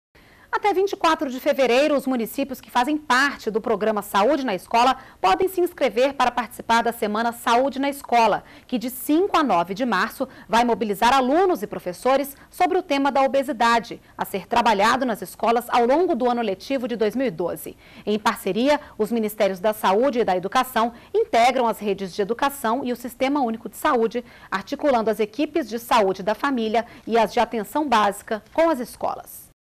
Abertas até 24 de fevereiro inscrições para Semana Saúde na Escola com tema obesidade
NBR Notícias - 22.02.11: Até 24 de fevereiro, os municípios que fazem parte do Programa Saúde na Escola podem se inscrever para participar da Semana Saúde na Escola, que de 5 a 9 de março vai mobilizar alunos e professores sobre o tema da obesidade, a ser trabalhado nas escolas ao longo do ano letivo de 2012. Em parceria, os Ministérios da Saúde e da Educação integram as redes de educação e o Sistema Único de Saúde (SUS), articulando as equipes de saúde da família e as de atenção básica com as escolas.